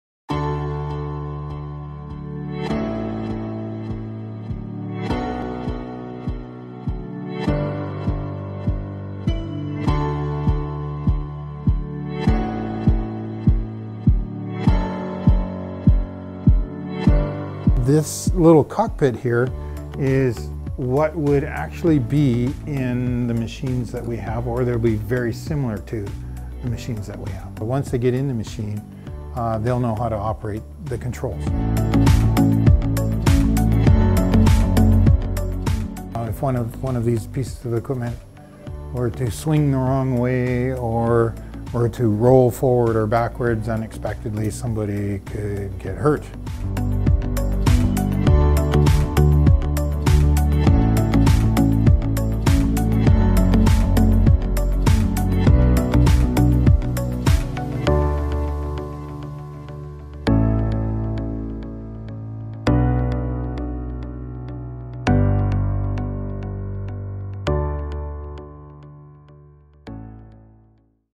Heavy Mechanic Simulators at BCIT's Annacis Island Campus
Students of BCIT's heavy mechanical trades foundation are using simulators to familiarize themselves with heavy equipments. Our reporter, Dylan Grant, caught up with an instructor from BCIT to talk about the new technology.